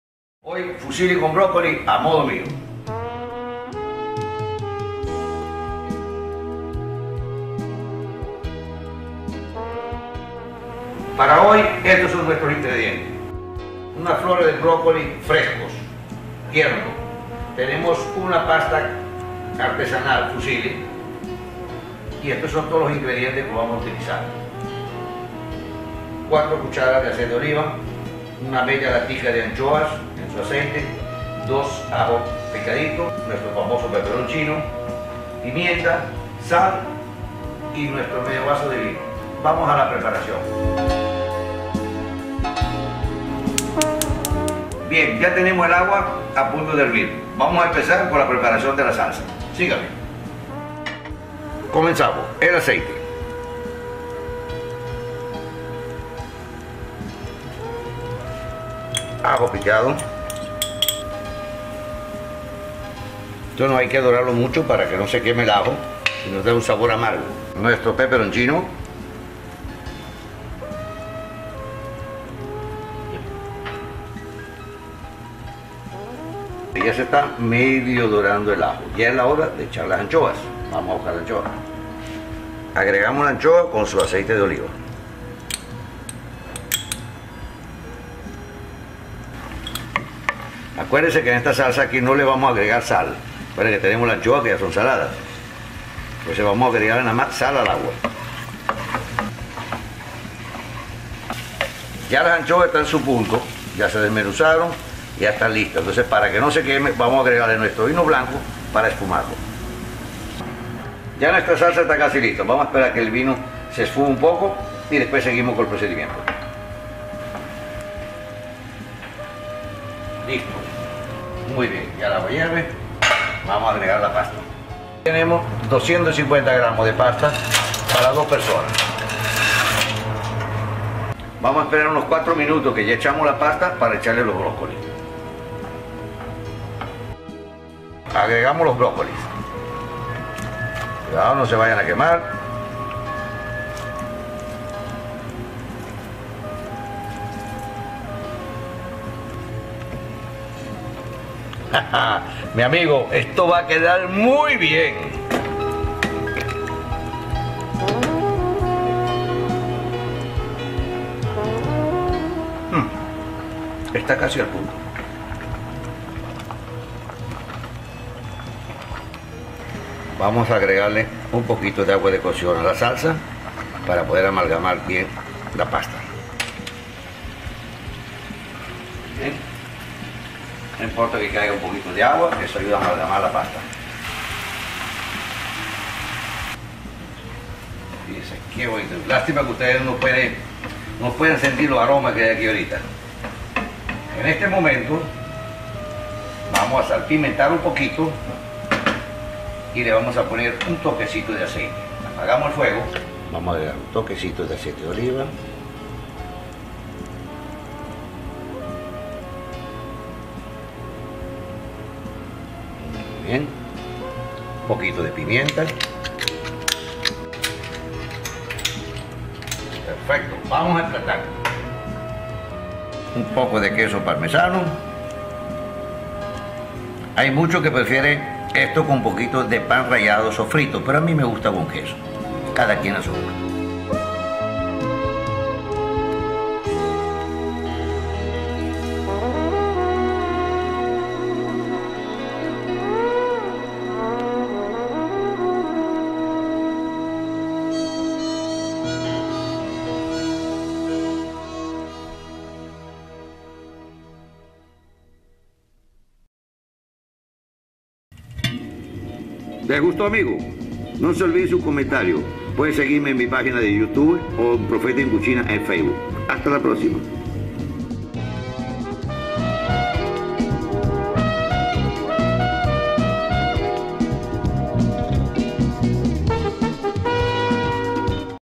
Fusilli con broccoli
Hola amigos, hoy otra receta muy rápida, muy sencilla, debo decir que a mi familia le gusta mucho, pienso que tiene la combinación perfecta de sabores, lo salado de las anchoas, la frescura del broccoli, ese toque de picante, todos los sabores se mezclan de una manera genial para hacer de algo muy simple, sublime.

Solo sigan esta simple receta, no se arrepentirán.